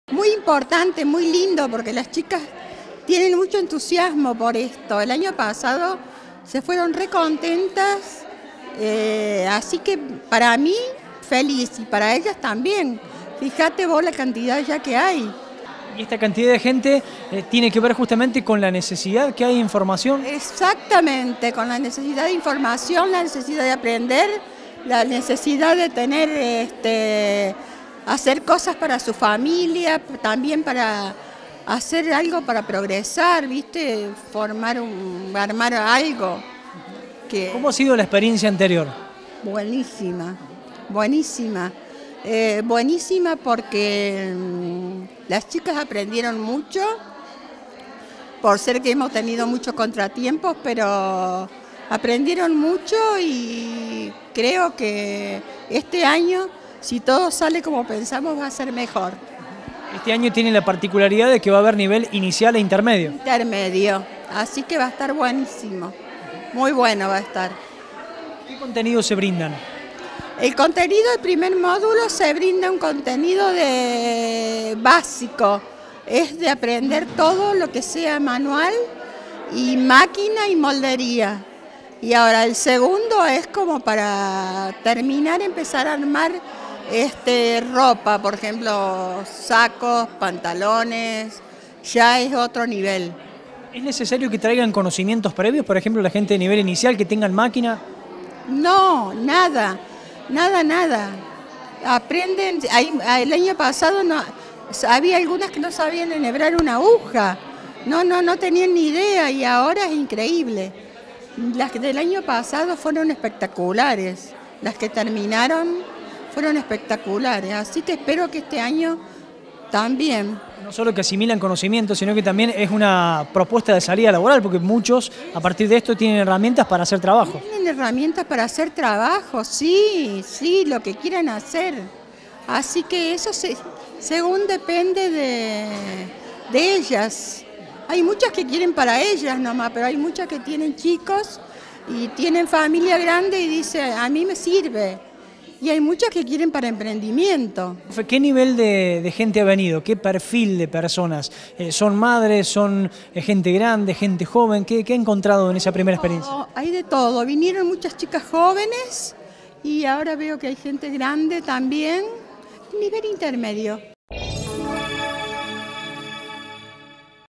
INSCRIPCIONES PARA EL TALLER DE DISEÑO Y CONFECCIÓN DE INDUMENTARIA EN HIGUERAS
MARÍA DEL CARMEN MEDINA – DOCENTE
Facebook.com/uniriotv
Twitter.com/uniriotv